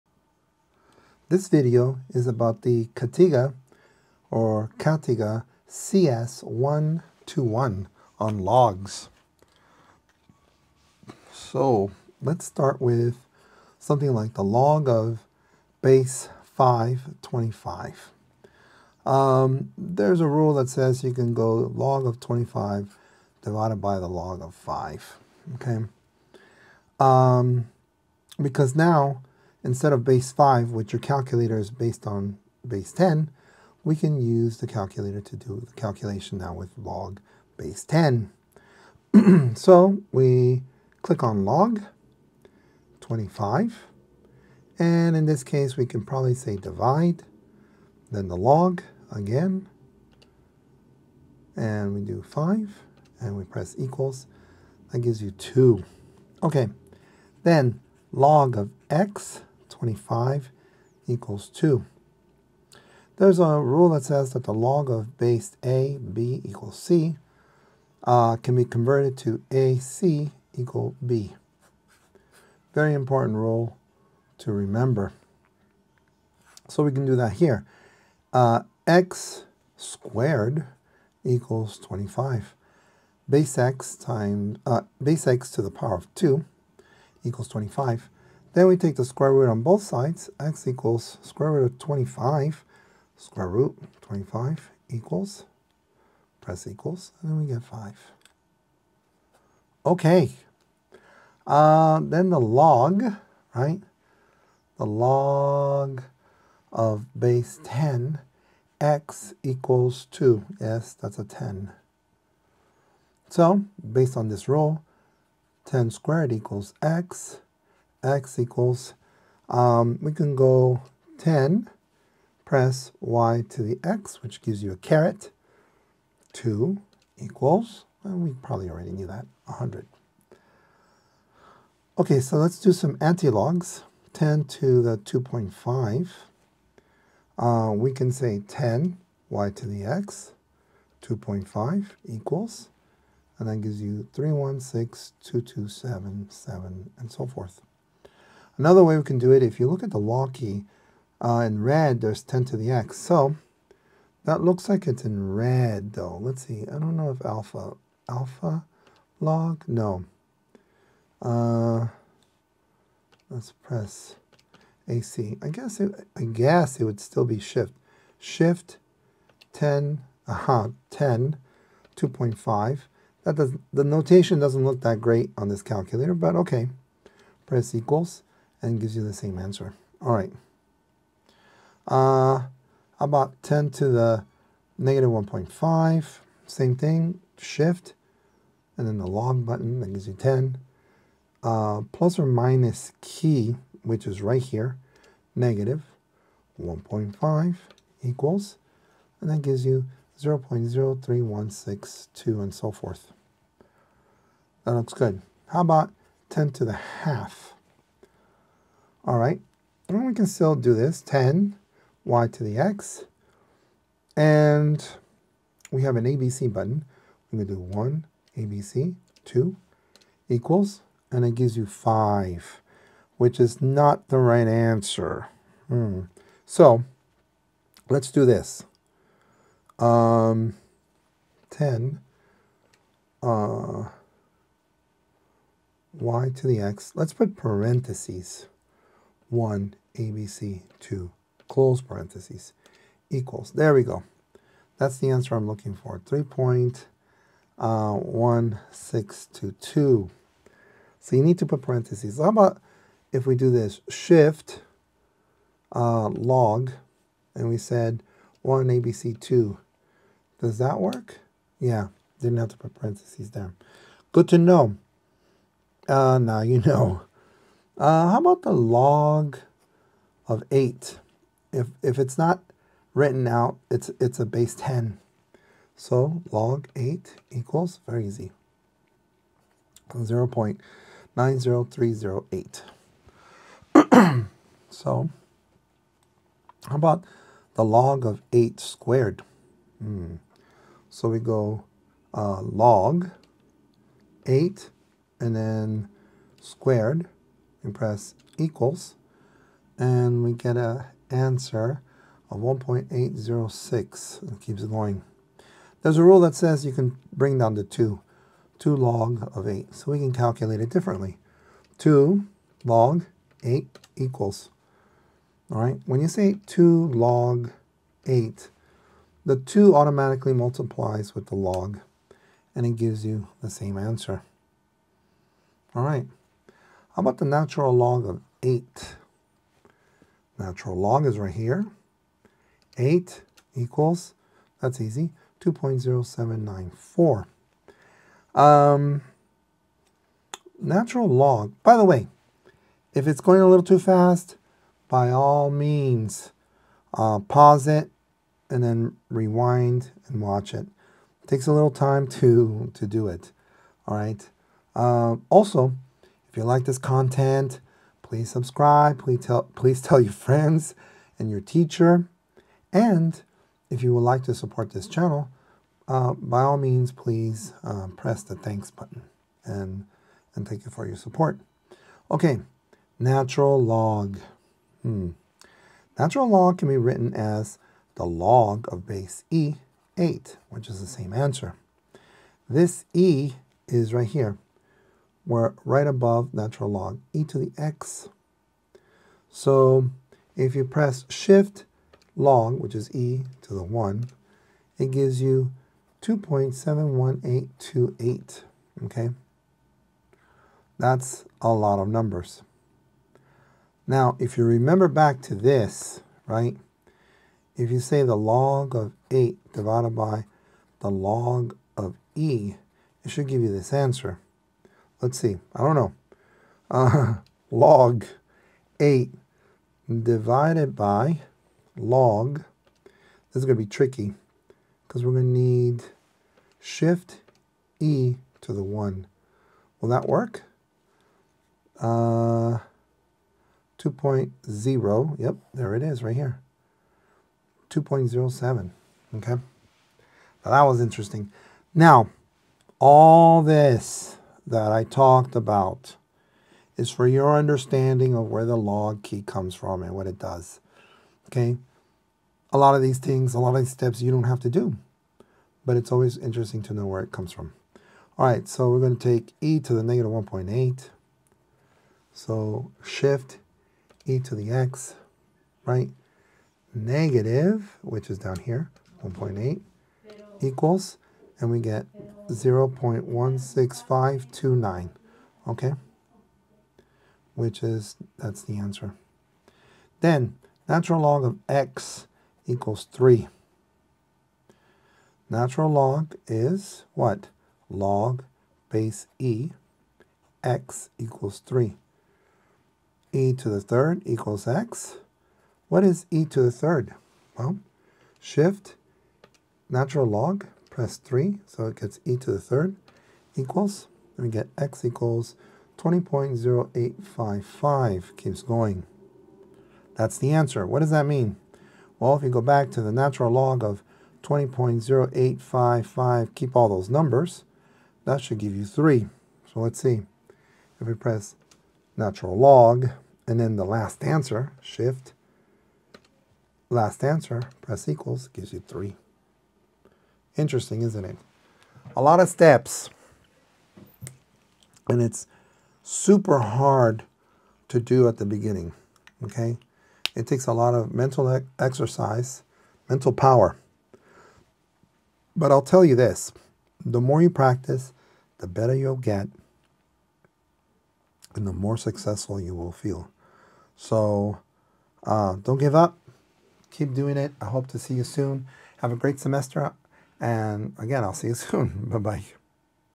CATIGA CS-121 Tutorial: Logarithms, Natural Logs, and Antilogs
In this video, I demonstrate how to use the CATIGA CS-121 calculator to solve logarithmic functions, including logarithms, natural logarithms, and antilogarithms. This tutorial is perfect for students and anyone looking to understand how to apply these functions on the CATIGA CS-121. I provide a clear, step-by-step approach to help you follow along easily. Whether you’re preparing for exams or need a quick refresher, this guide will give you confidence in using the CATIGA CS-121 for logarithmic calculations.